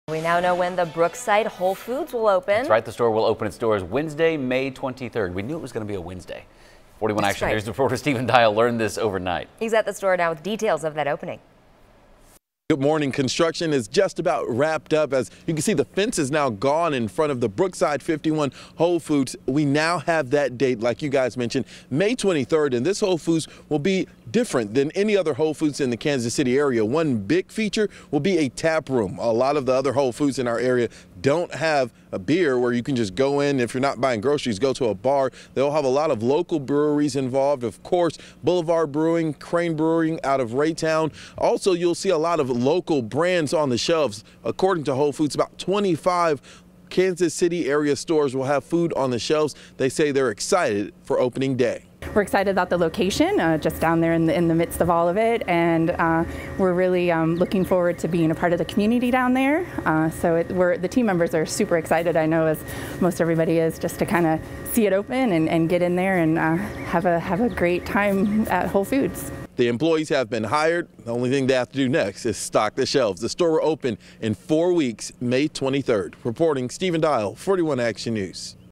Whole Foods Market Kansas City to open May 23
Whole Foods has announced when it will open its first Kansas City, Missouri, store at 301 E. 51st Street. The grand opening for the nearly 50,000-square-foot store will be on Wednesday, May 23 at 9:00 a.m.